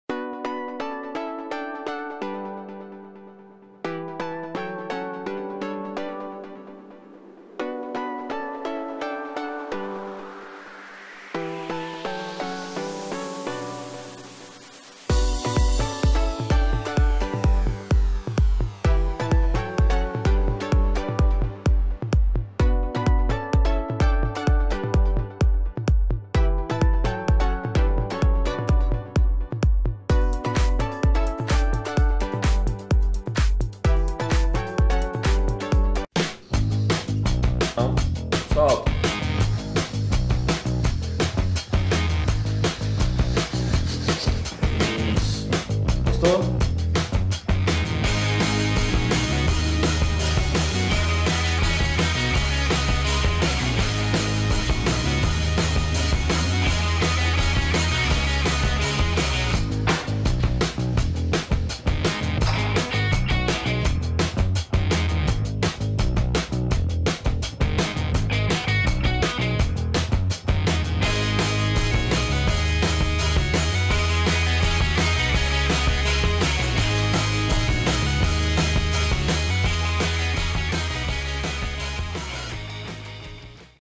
Super Carrinho Movido a Balão - 3ª Lei de Newton
Projeto de um super carrinho movido a balão onde é demonstrado a aplicação da 3ª lei de Newton.